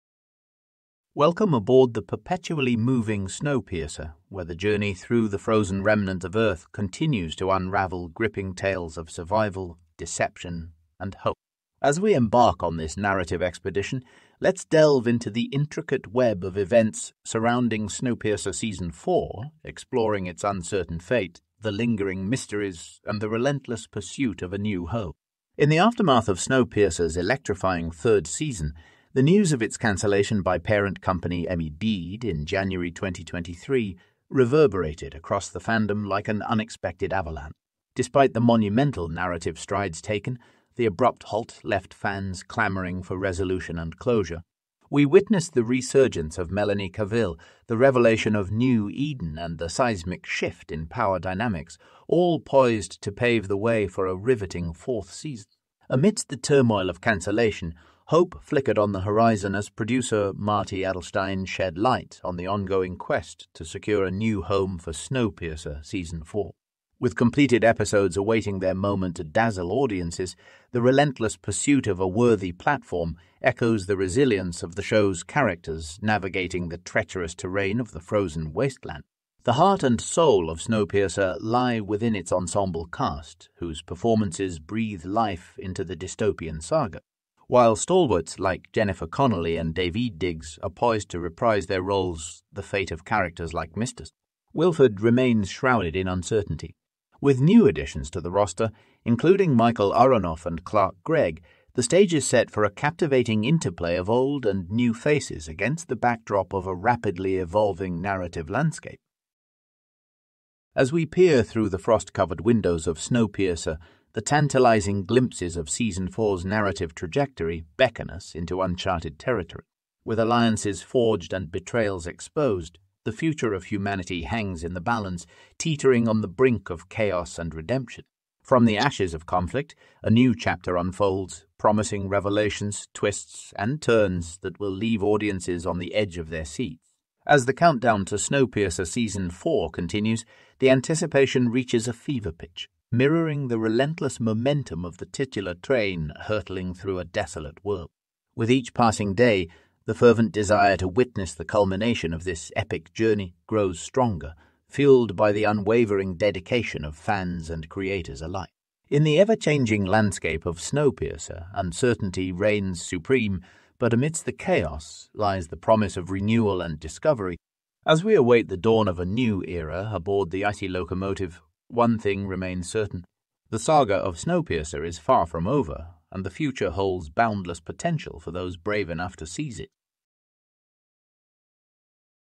Snowpiercer Season 4: Confirmation & Everything We Know
Welcome to the Snowpiercer Season 4 extravaganza! Join us on an epic journey through the frozen wasteland of Earth as we unravel the mysteries, shocks, and surprises that await aboard the perpetually moving train.

In this comprehensive guide, we delve into the aftermath of Snowpiercer's cancellation shockwave, the relentless quest for redemption, and the tantalizing hints of what's to come in the upcoming season. Get ready to explore the fate of your favorite characters, the twists and turns of the narrative, and the pulse-pounding anticipation that surrounds the search for a new home for this beloved dystopian saga.

From cast updates to story predictions, we leave no stone unturned in our quest to bring you the latest and greatest insights into Snowpiercer Season 4. So grab your ticket, buckle up, and get ready for the ride of a lifetime!

The future is uncertain, but one thing's for sure: the journey aboard Snowpiercer is just getting started. 👇